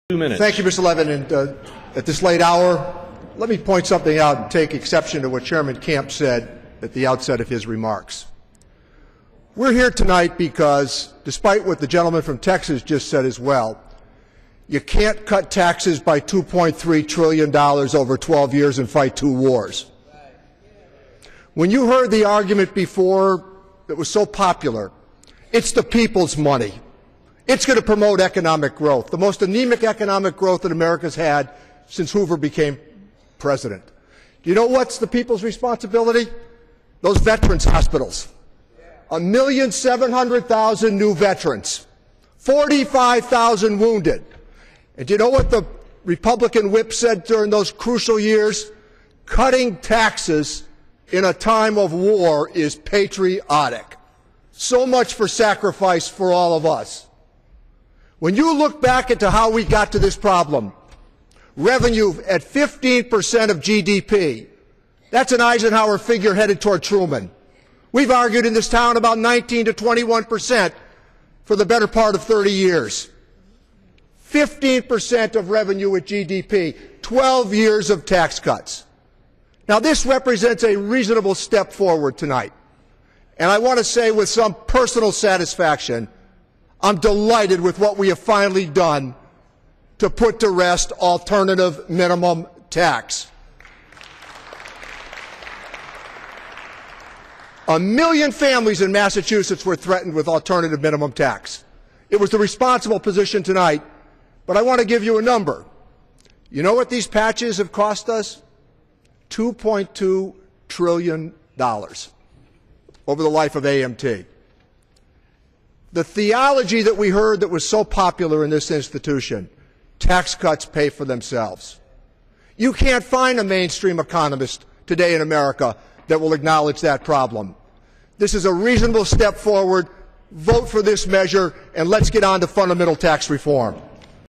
Congressman Richard E. Neal Speaks Out in Support of Bipartisan Tax Package
Congressman Richard E. Neal speaks on the floor of the U.S. House of Representatives on Tuesday, January 1, 2013